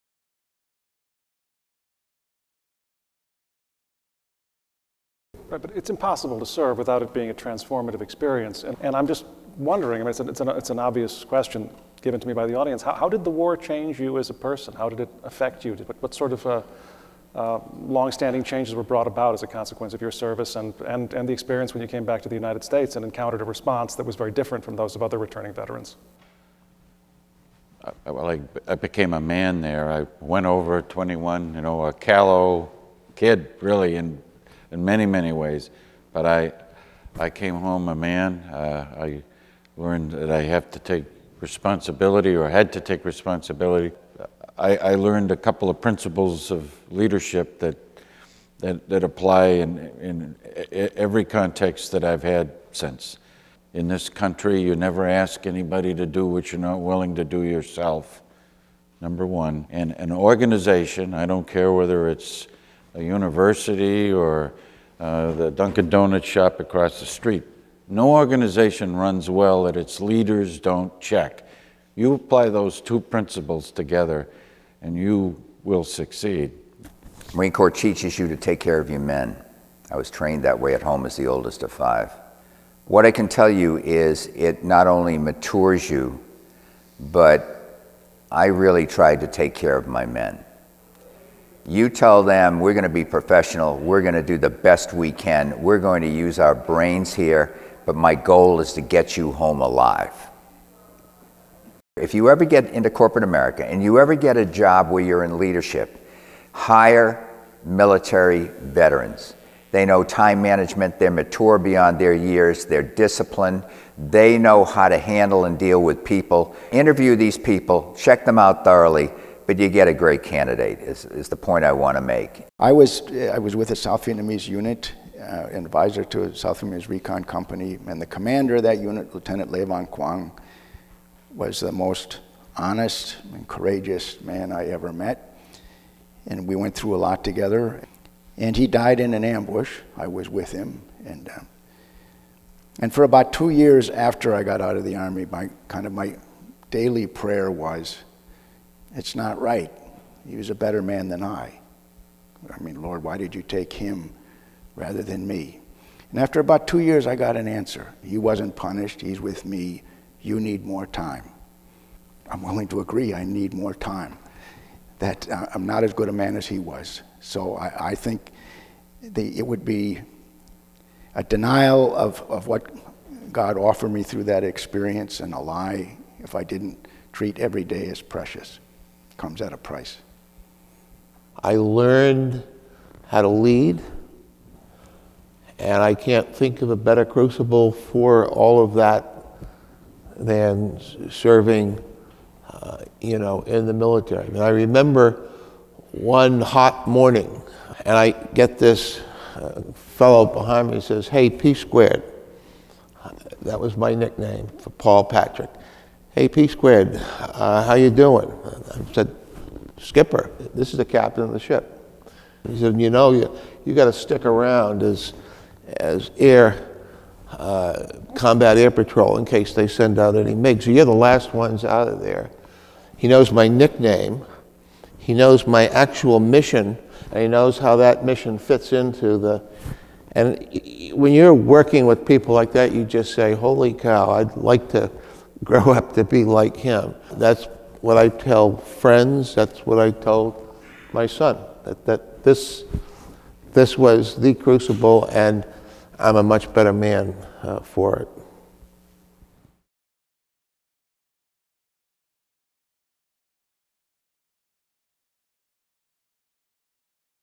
"Crucible"
On November 11, 2014, some 40 years after the U.S. withdrawal from Vietnam, four Boston College alumni met before an audience in Gasson 100 to talk about the experience of combat in Southeast Asia and the lasting effects of that time: Paul Daley ’63, a retired lawyer who flew 212 combat missions as a naval officer; James Huse Jr. ’65, former inspector general for the Social Security Administration whose Army tours of duty were as an infantry officer and special intelligence officer; Paul McNellis, SJ, Ph.D.’93, a member of the Boston College philosophy department who was an Army infantry officer advising South Vietnamese military units; and Larry Rawson ’63, a commentator for ESPN who served as a Marine intelligence officer and artillery forward observer.

Their discussion was moderated by Seth Jacobs, history professor at Boston College and the author of Cold War Mandarin: Ngo Dinh Diem and the Origins of America’s War in Vietnam, 1950–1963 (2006).

A production of Boston College Magazine's @BC Studio

Producer: Ravi Jain. Video: Michael Archambault, Ravi Jain.